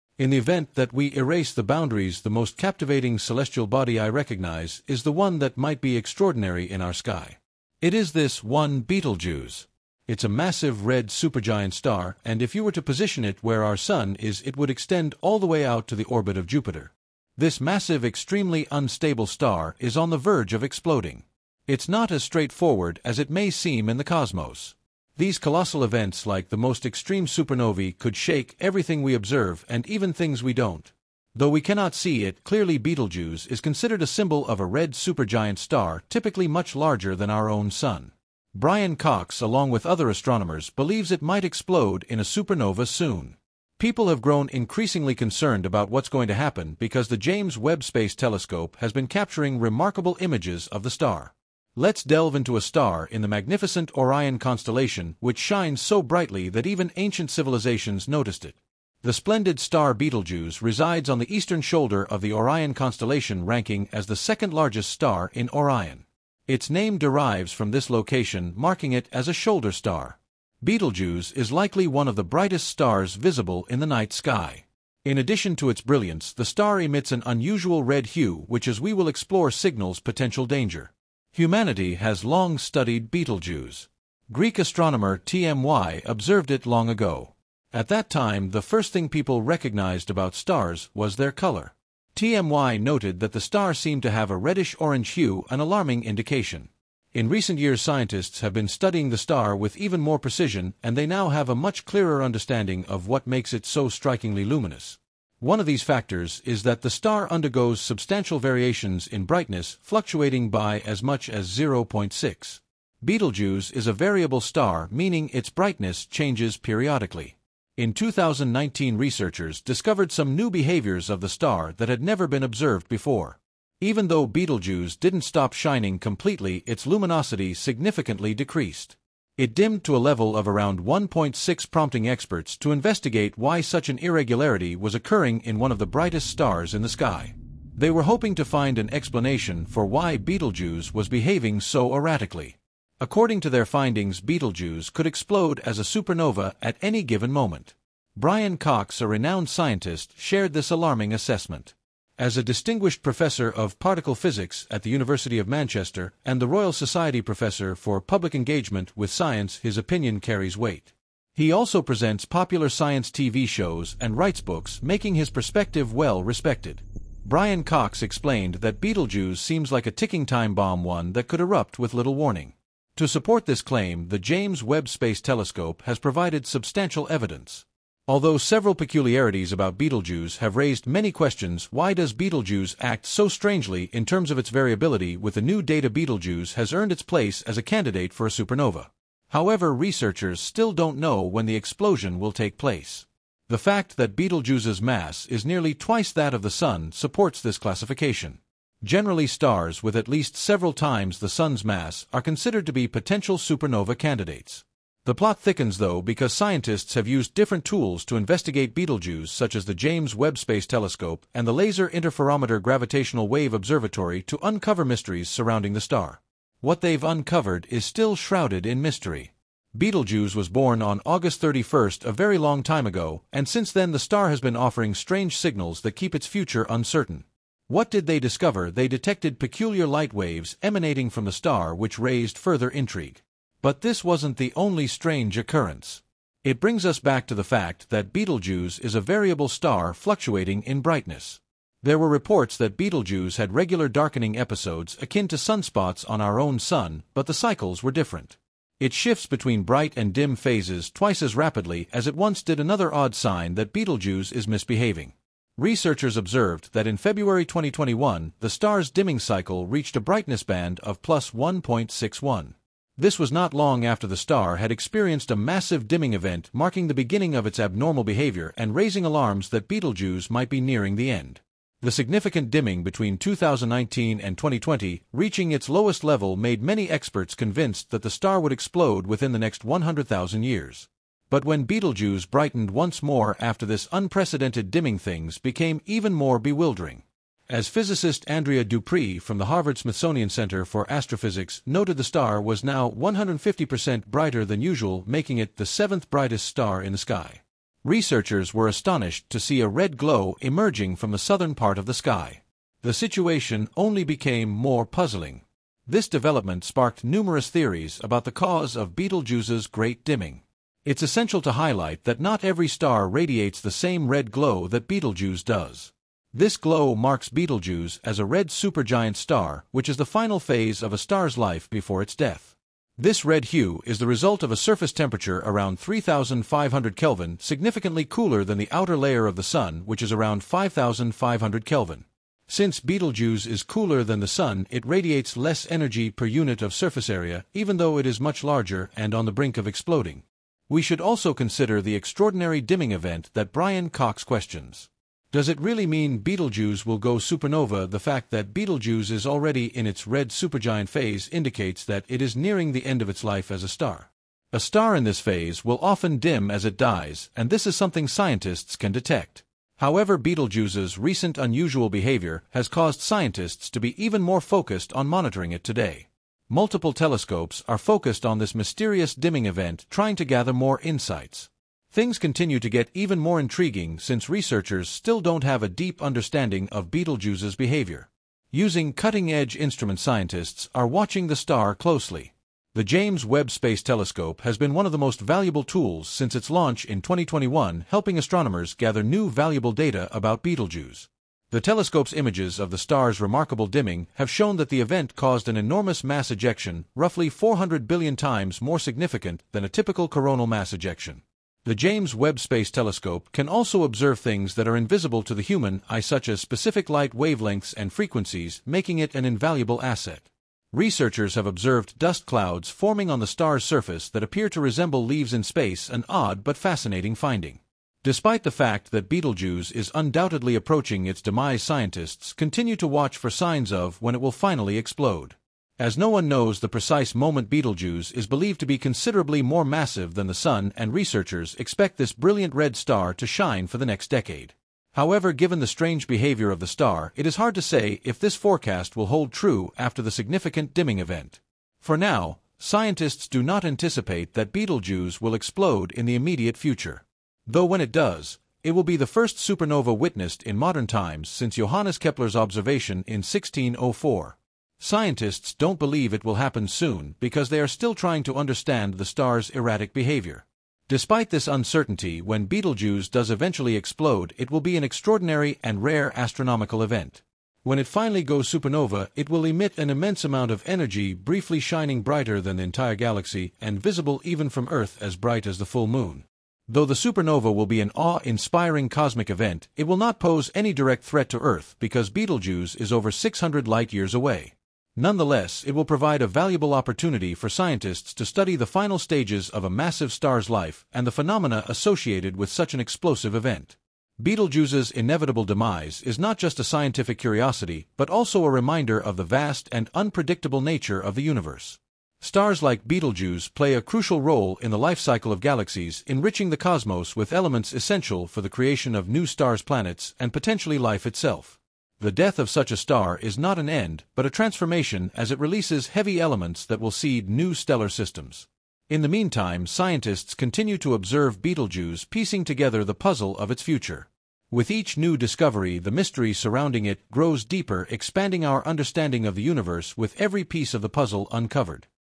Countdown Begins: Brian Cox Reveals imminent Betelgeuse Supernova.
🌌 Get ready! The countdown begins as Brian Cox reveals the imminent Betelgeuse supernova! Join us as we explore the dramatic unveiling of this cosmic event and its profound implications for astronomy. Dive into the excitement with one of the world's foremost astrophysicists and witness history in the making. Don't miss out on this extraordinary journey through space and time!